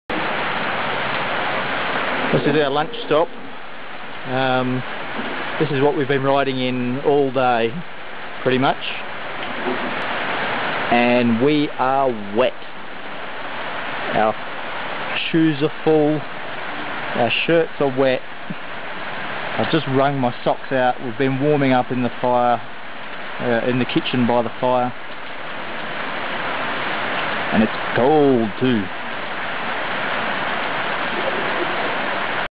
Rain in Bhutan
We rode our Royal Enfield Motorcycles through this all day